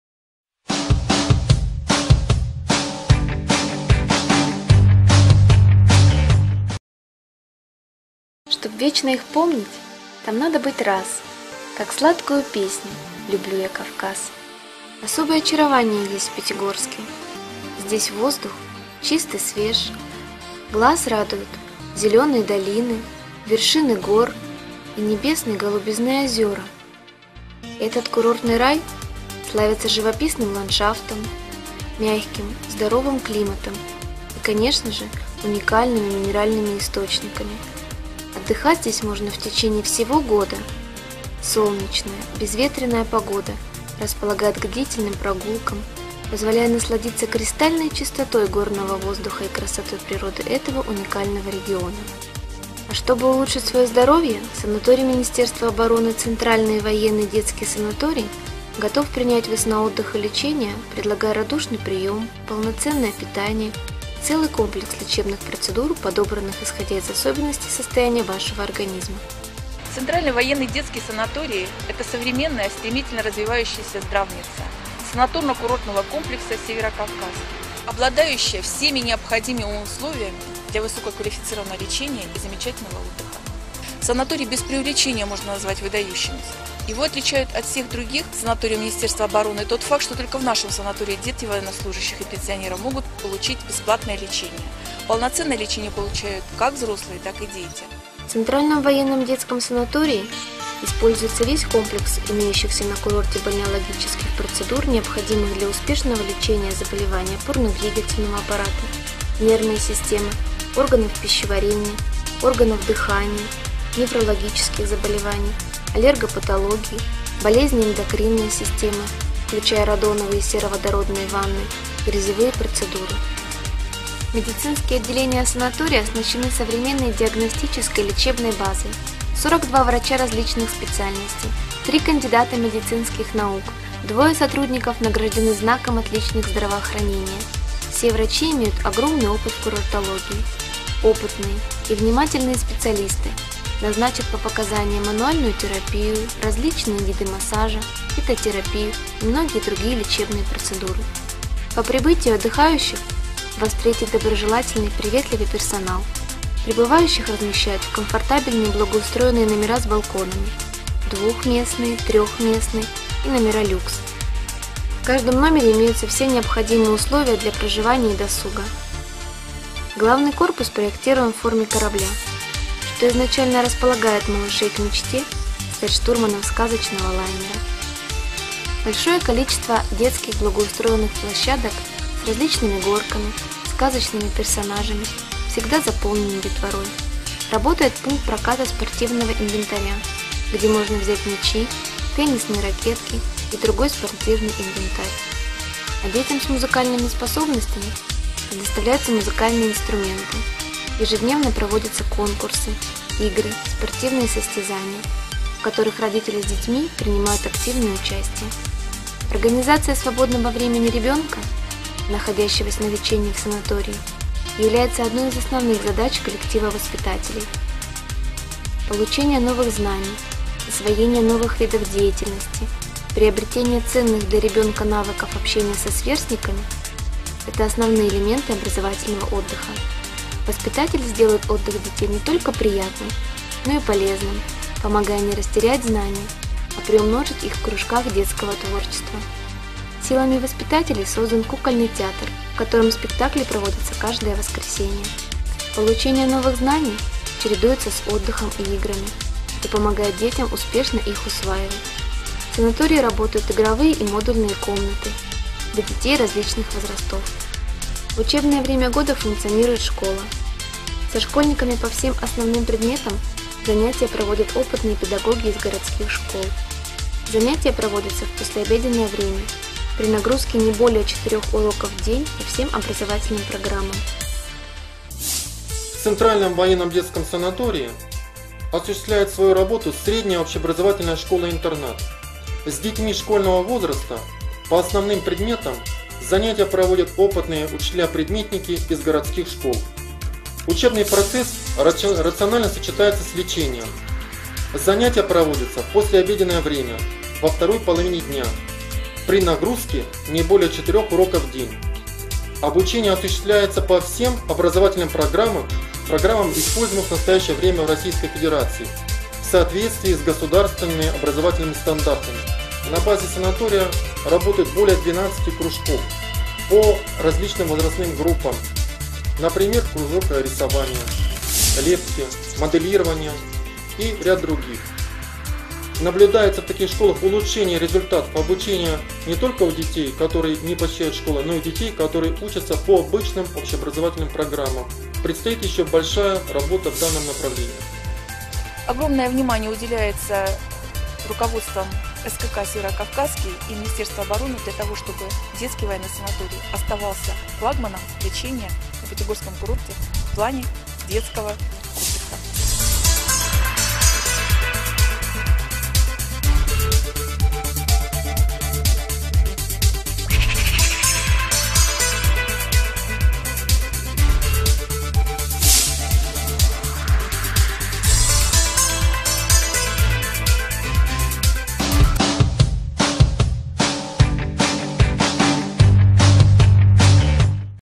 Санаторий Центральный Военный Детский (Ставропольский край, город Пятигорск)
В Пятигорске, у подножия горы Машук находится санаторий Центральный Военный Детский. Совсем недалеко – бювет воды «Ессентуки новая», на окраине города – рукотворное озеро, пригодное для летнего купания. Всего в 2 км от санаторных корпусов находится лесной массив Дубровка. На территории, кроме жилых корпусов и лечебницы, есть школа, где детвора может оздоравливаться без прерывания учебы.